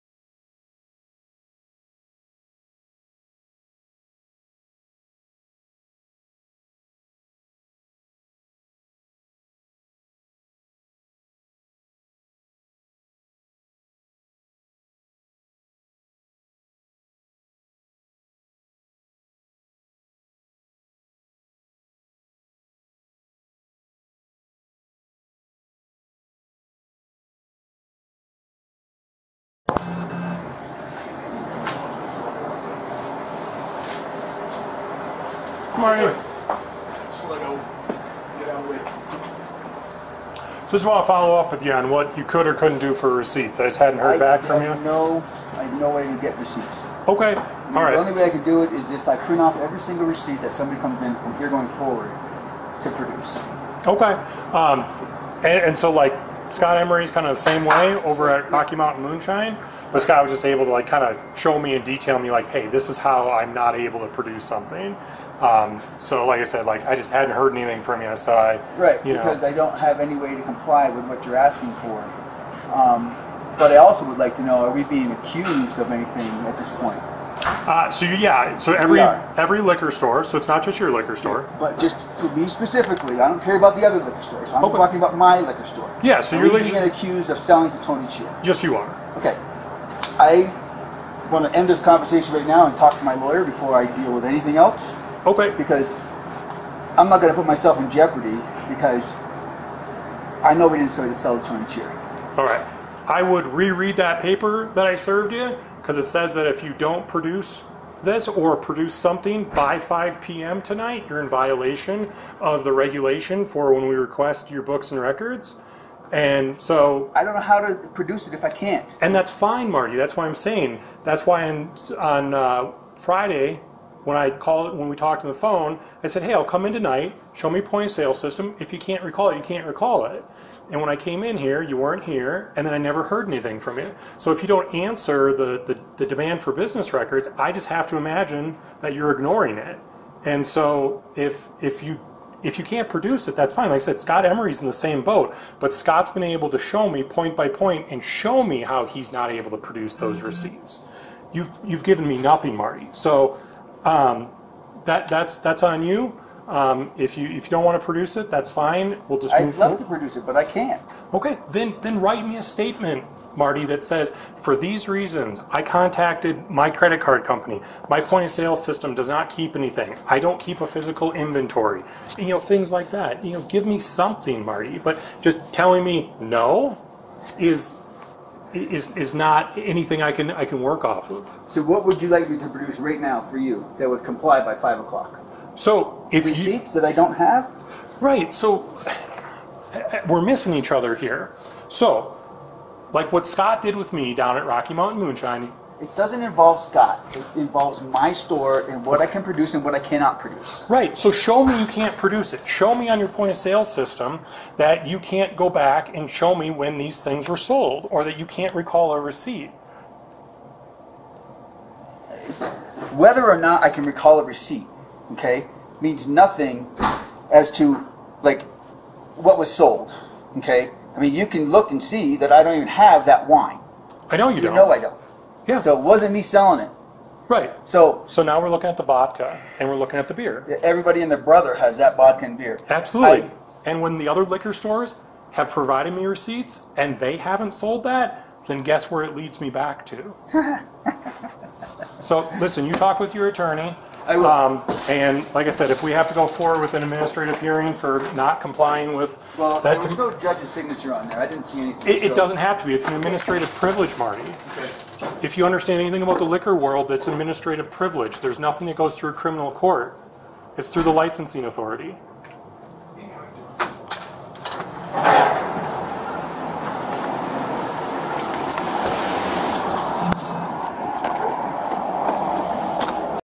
Part 2 Winter Park Wine being RAIDED and illegally collecting personal data
This is part 2 of the Winter Park Fraser Police collecting personal data without a warrant. officer Jeff got a promotion because of this illeagal activty. winterparkco coruption This is store was visited 4 times even after they had the suspect in a different store. They had a suspect on camera in a different store and still went after this Owner.